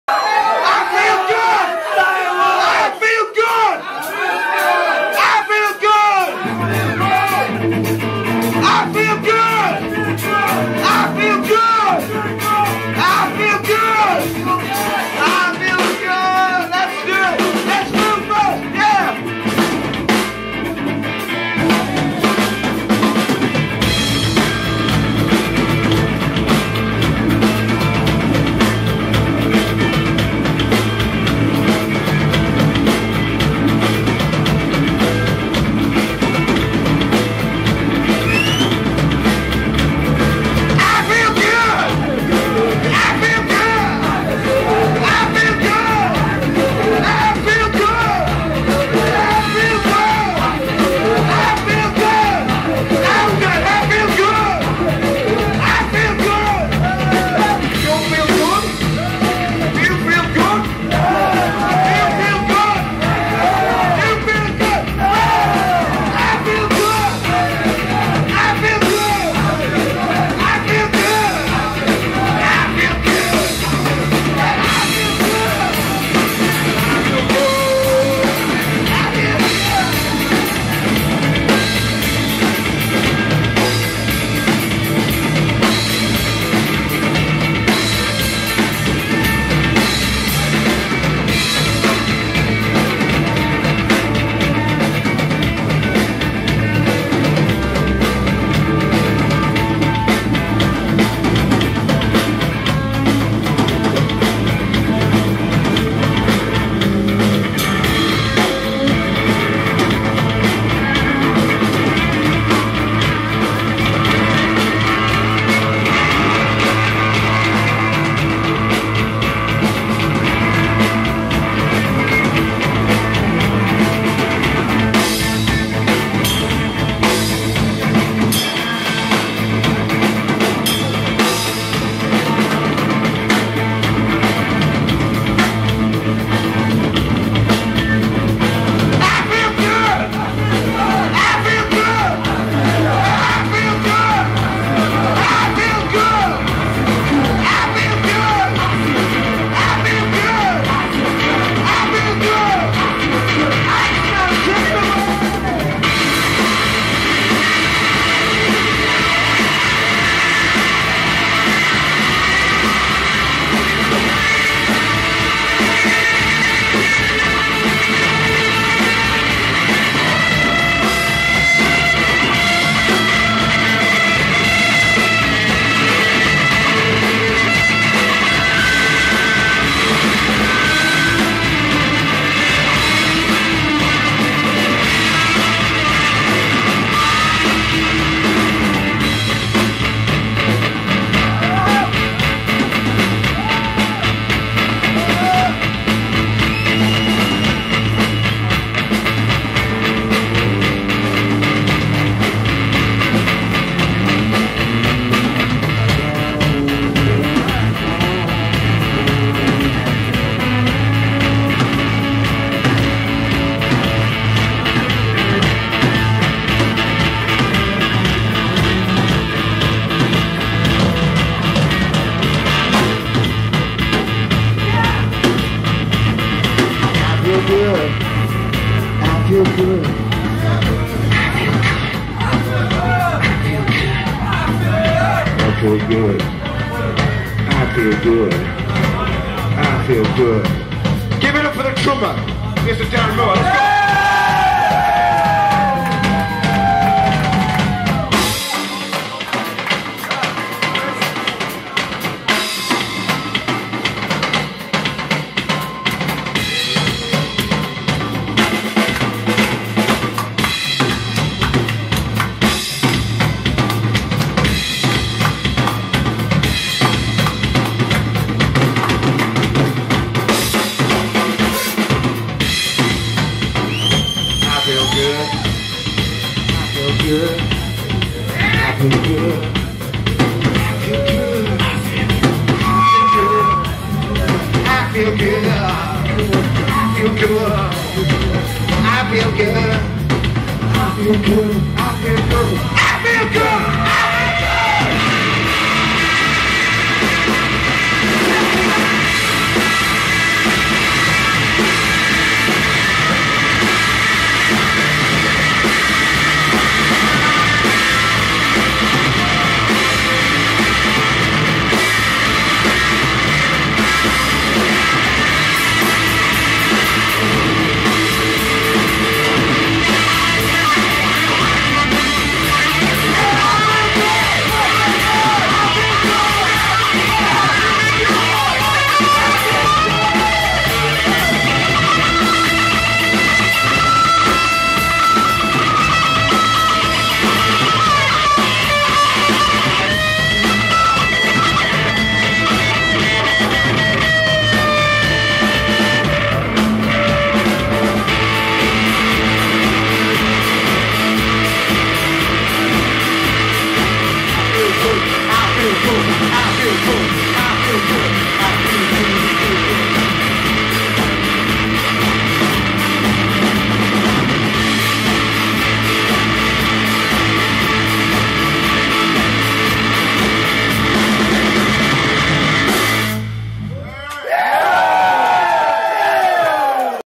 The Magick Godmothers (Bizzarnica pri Mariči - 18.07.2016 - Metelkova mesto)
Koncert skupine The Magick Godmothers pri Mariči.

Snemano z Canon G16.